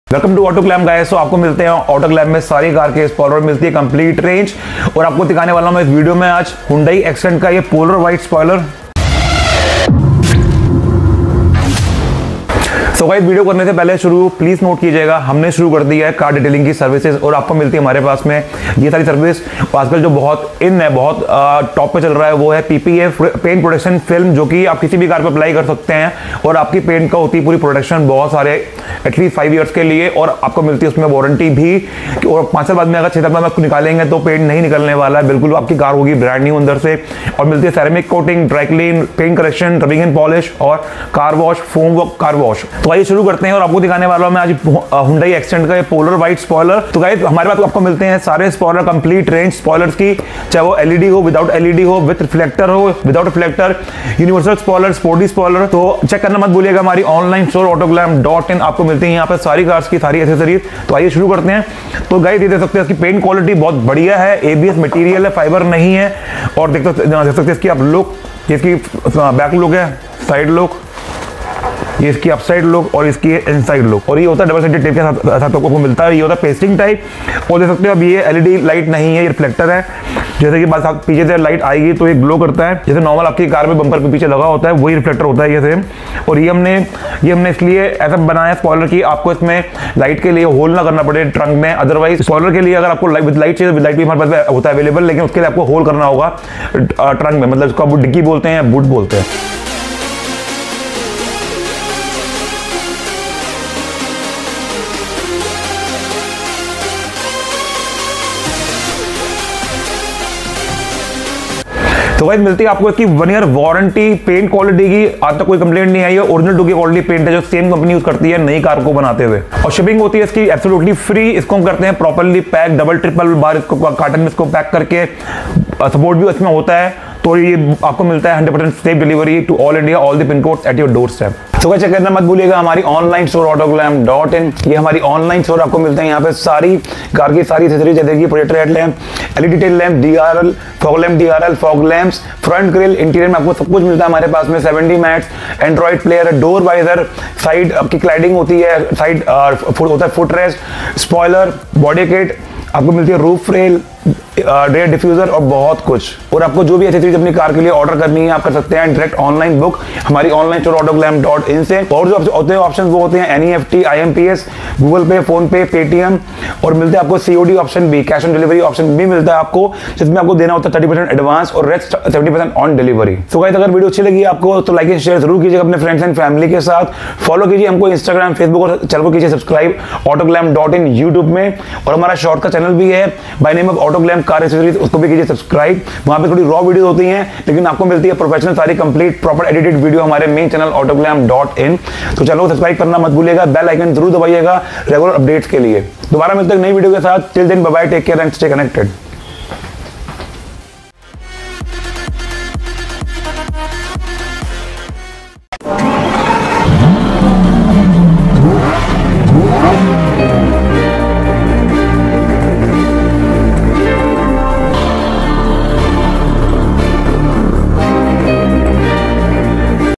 Hyundai Xcent Spoiler |सबसे आसान तरीका 100% |3M tape Spoiler fitting |No clickbait🚫Cheapest spoiler
FOLLOW US -

ALSO CHECK -